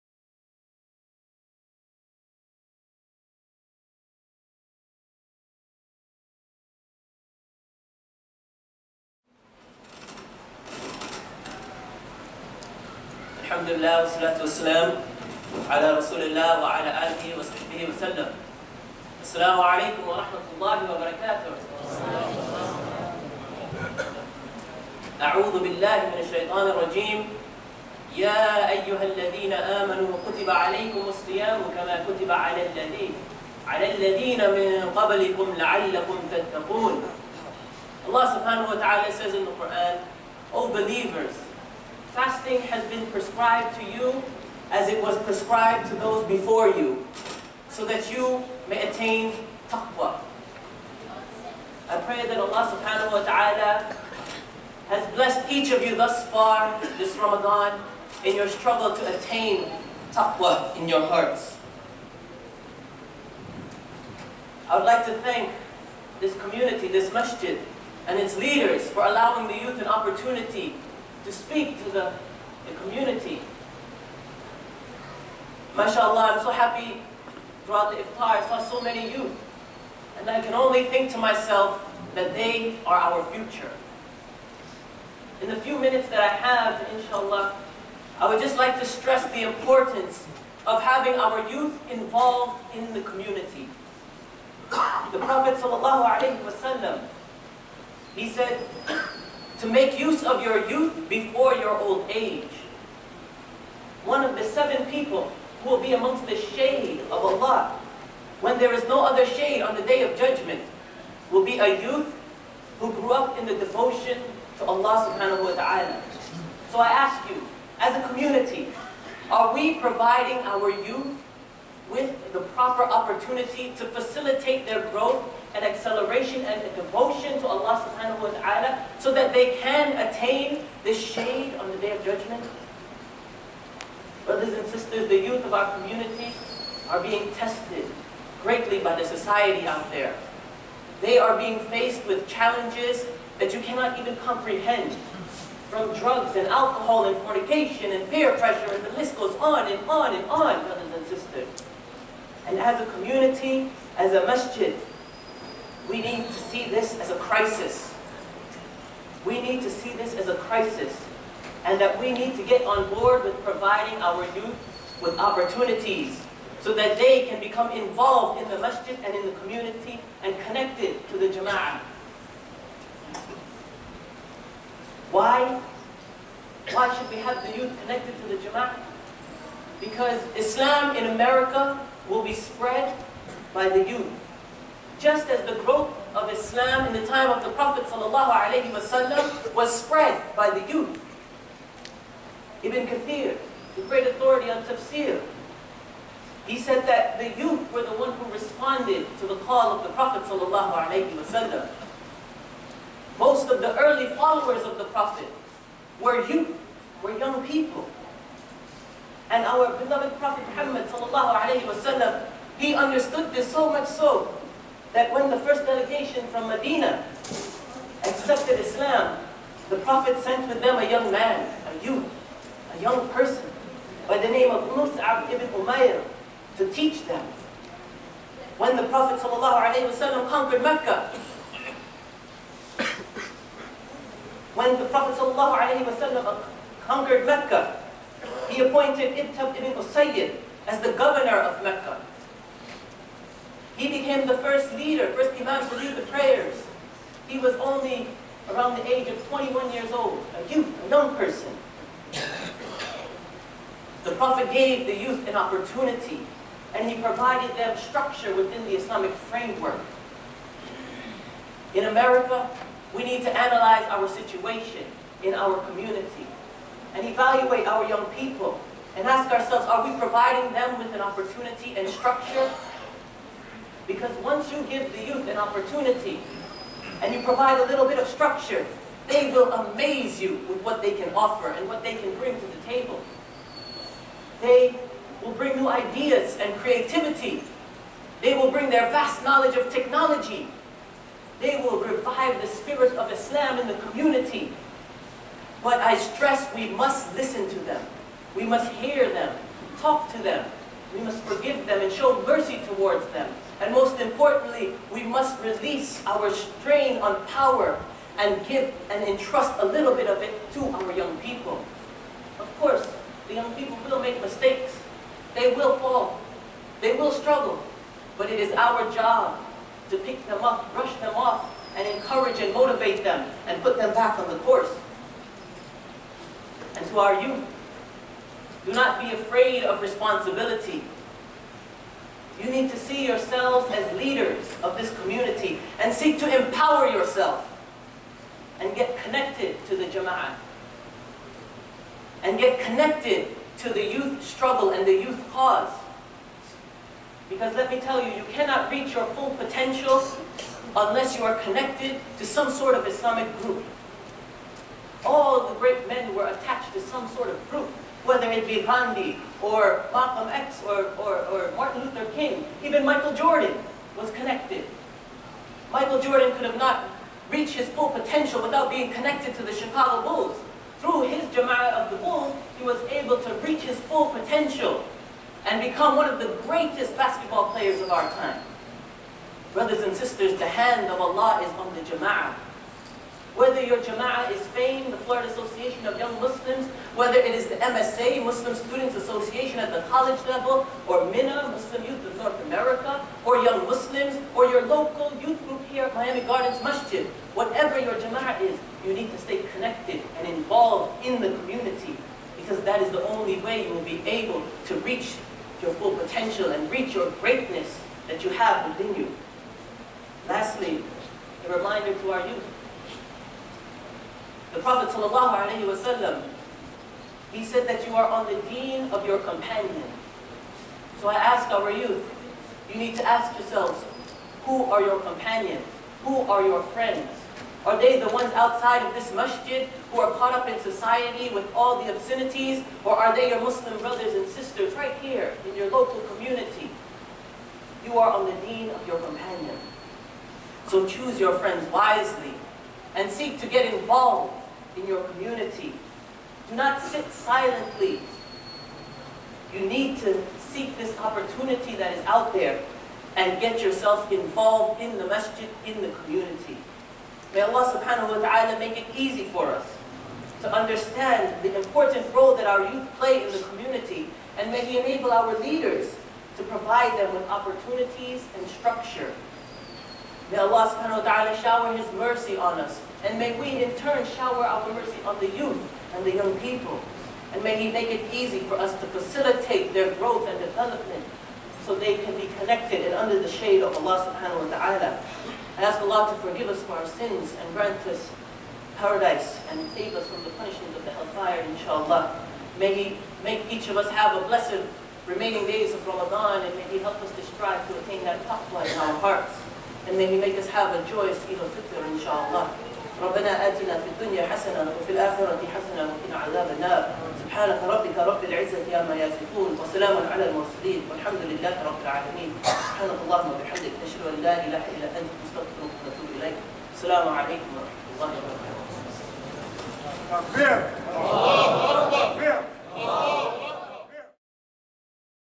Our Youth, FAYM @ MGM, Raa'id Khan
Raa'id Khan of FAYM (Florida Association of Young Muslims) gives a short talk at MGM (Miami Gardens Masjid) on September 12, 2009. He speaks on the role of our Muslim youth in the community.